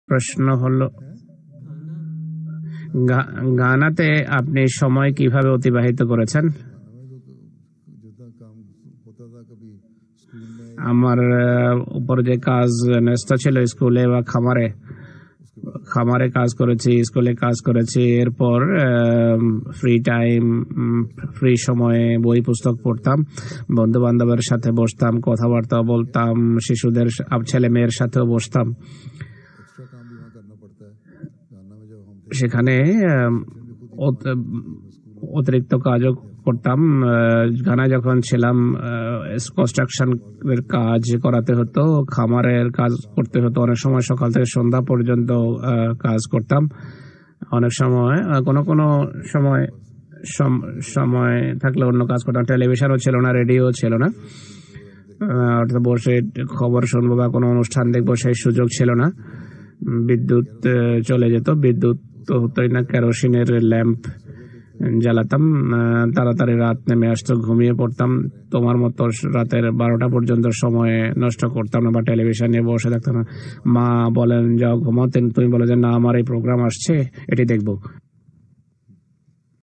হুযুর, ঘানাতে আপনি কীভাবে সময় অতিবাহিত করেছিলেন?
গত ৯ ফেব্রুয়ারি ২০২০ ইং তারিখে হুযুর আনোয়ার হযরত মির্যা মাসরুর আহমদ (আইঃ) ওয়াকফেনও আতফালদের একটি ক্লাস নেন।